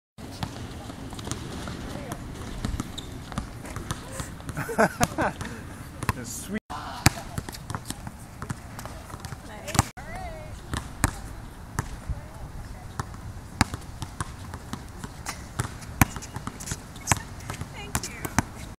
summer afternoon in the park
playing horse.  ball around the torso followed by a layup.  this was after a few rounds including left-handed skyhooks, half-court hail marys, and three-pointers from the baseline.